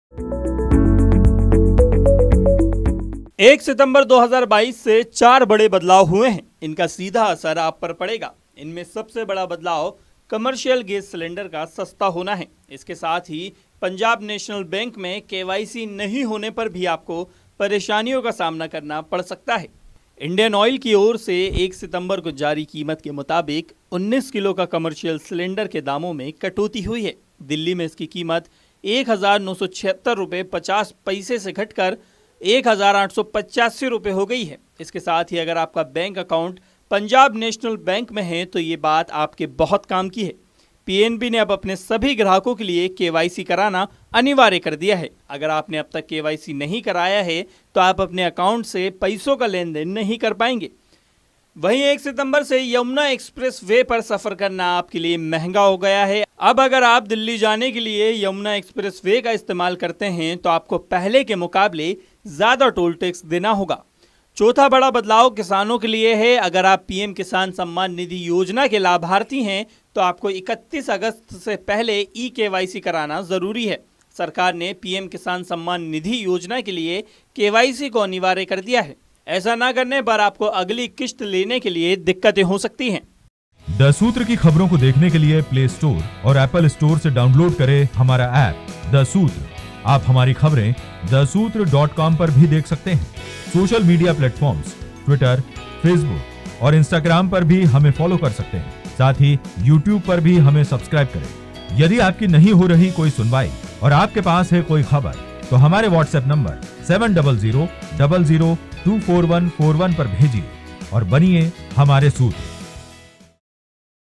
1 सिंतबर से हुए यह 4 बड़े बदलाव आपकी जिंदगी पर असर करेंगे
1 सिंतबर 2022 से 4 बड़े बदलाव हुए हैं...इनका सीधा असर आप पर पड़ेगा...इनमें सबसे बड़ा बदलाव कॉमर्शियल गैस सिलेंडर का सस्ता होना है. इसके साथ ही पंजाब नेशनल बैंक में KYC नहीं होने पर आपको परेशानियों का सामना करना पड़ सकता है. आईये जानते हैं आज से क्या 4 बड़े बदलाव हुए हैं.

The Sootr
Atarangi Media house private limited
3, Indira Press Complex,